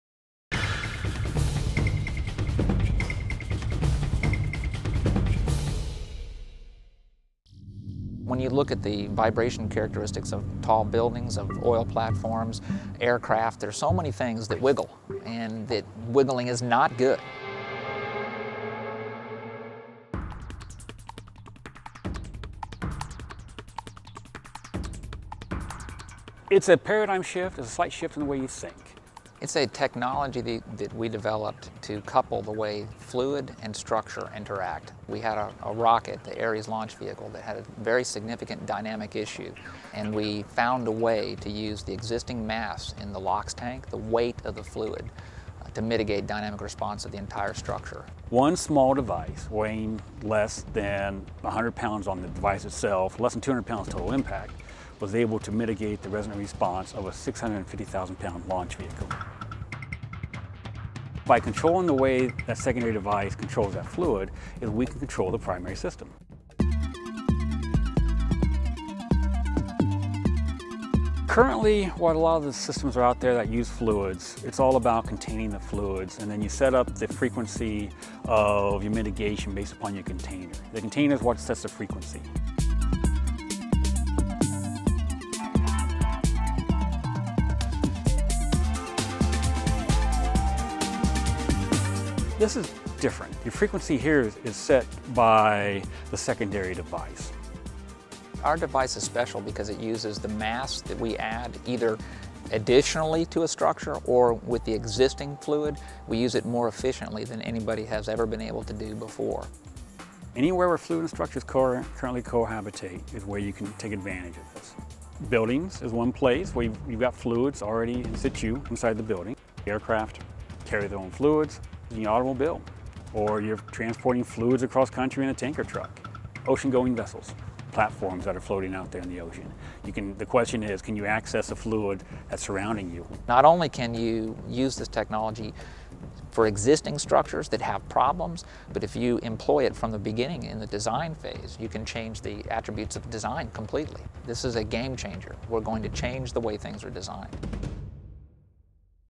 How NASA could earthquake-proof buildings
This is a brilliant technology that utilizes a structure's internal fluids to abate externally induced vibration. A NASA technology originally designed to stabilize rockets could now help buildings survive earthquake damage. The patented technology, called fluid structure coupling, uses simple physics to dampen potentially harmful shaking in structures. HECK Productions worked with NASA contractor RTI International to produce this informative look at a burgeoning technology.